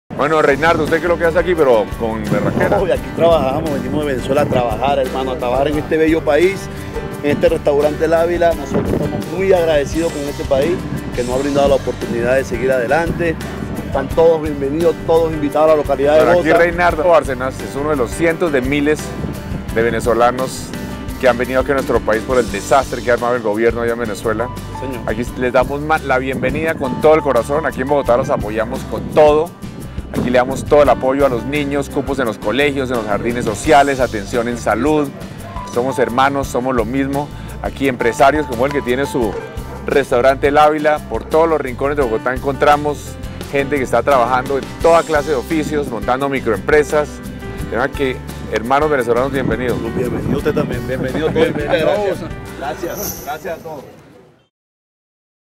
Enrique Peñalosa: Aquí apoyamos a los venezolanos
El alcalde de Bogotá, Enrique Peñalosa, deja en claro su apoyo a los venezolanos que llegan a la capital de Colombia.